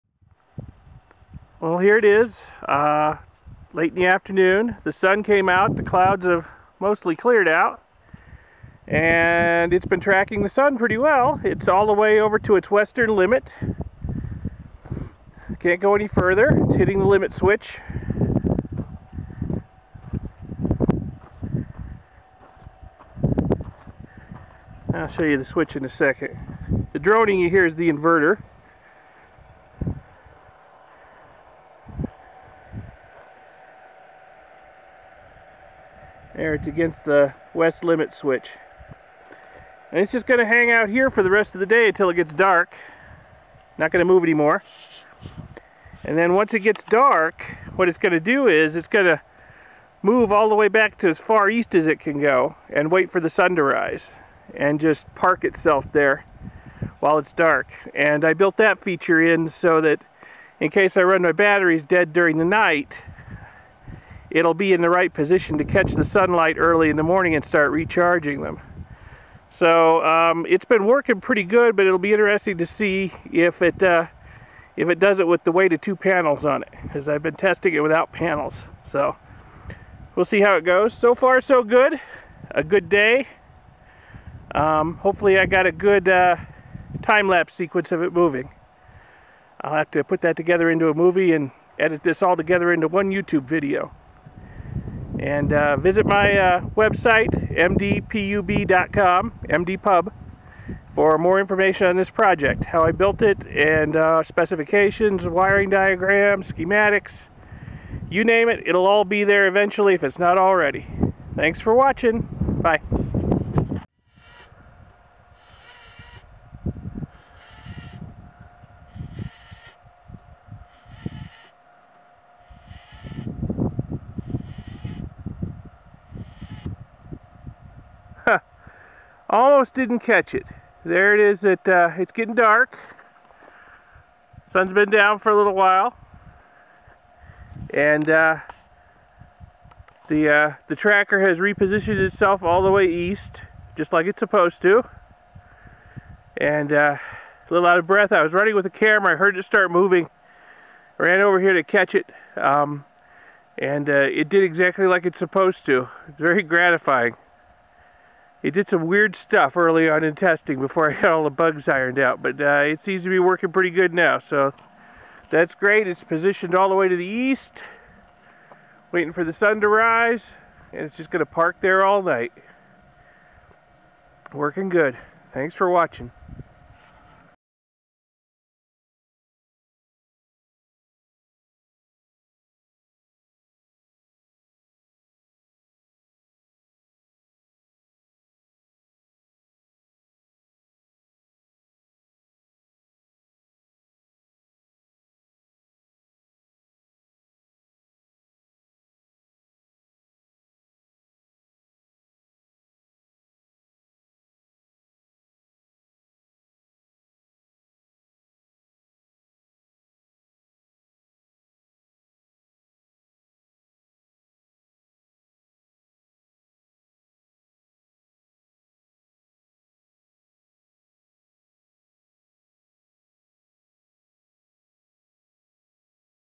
Solar tracker set up and running in Arizona
My solar panel tracker set up and running on my remote off-grid Arizona property. The tracker is based on an antenna rotator. You can learn more about this project at mdpub.com.